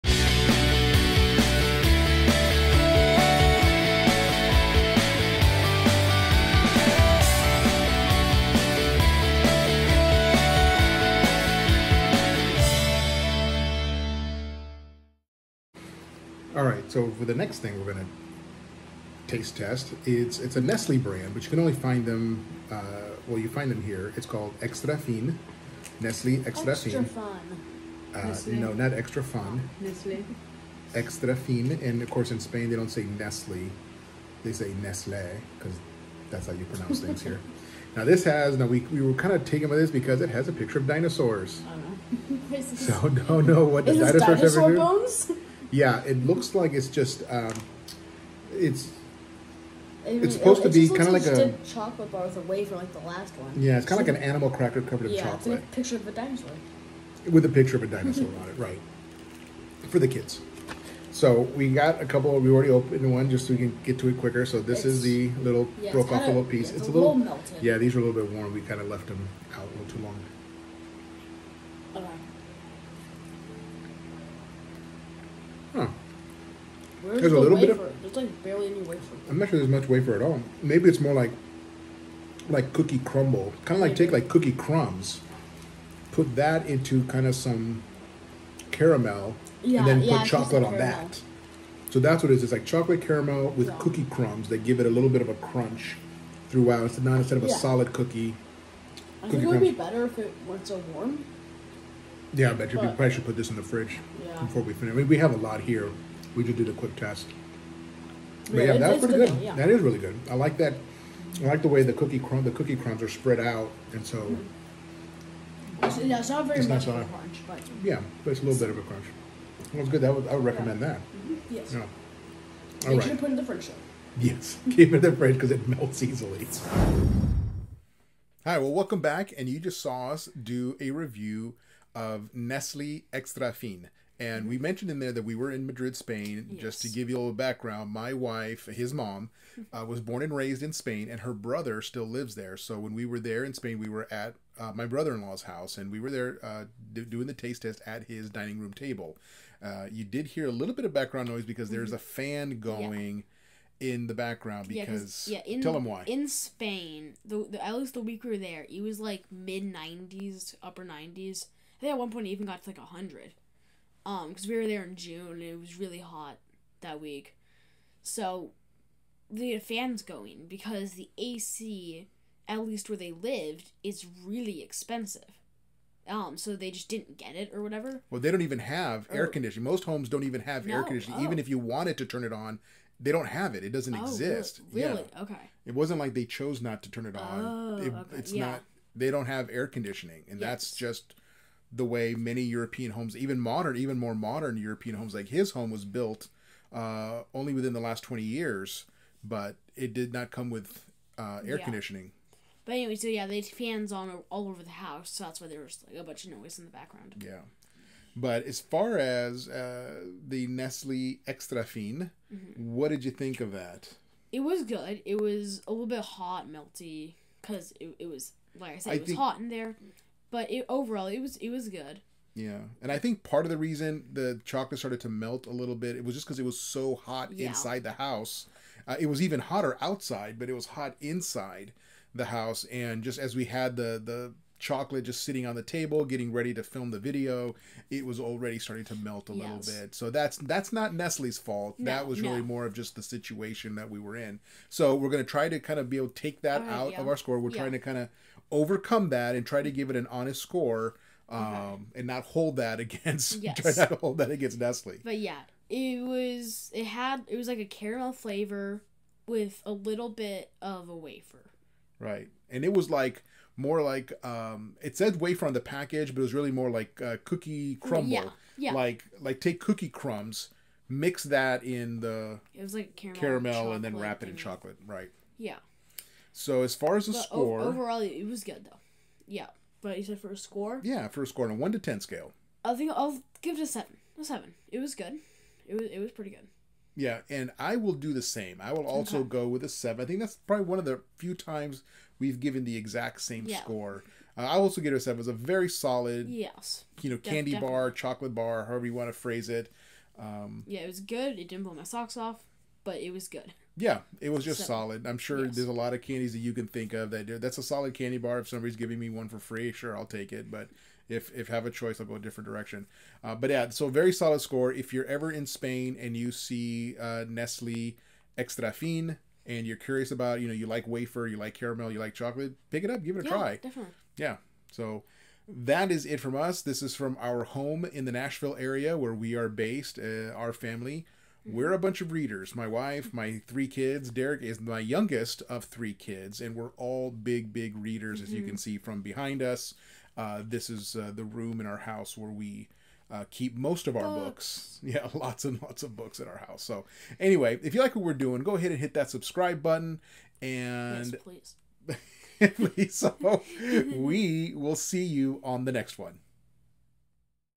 🇪🇸 Nestle - Extrafín (Spain) | Snack Review
We review a chocolate bar from Spain, Nestle Extrafín. It contained cookie crumbs inside of caramel, then wrapped in chocolate. We both really liked it.

Score:
Rick - 7
Derek - 7

0:00 Intro
0:16 Nestlé - Extrafín
2:07 Score